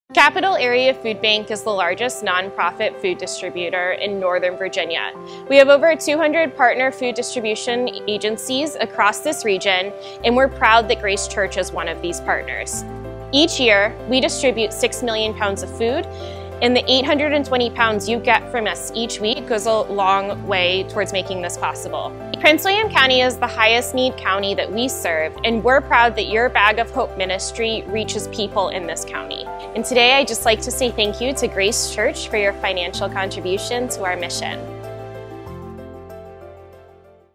Capital Area Food Bank Video
Serving our community is a priority so last week Grace Church provided 6,250 meals to those in need in the DMV.